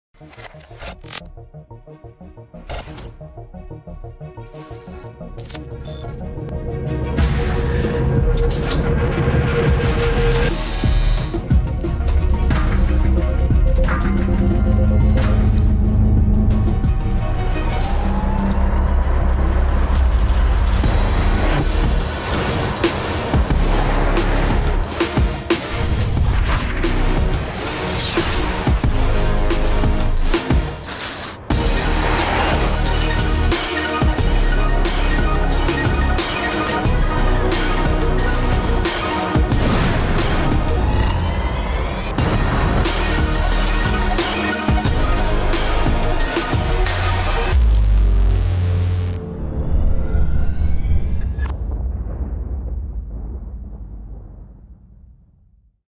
Lotus Evija Makes Dynamic Debut
Lotus Evija engineering prototype #2 in high-speed action on a private circuit. This film marks a landmark moment – the dynamic world debut of the Lotus Evija, the first all-electric British hypercar.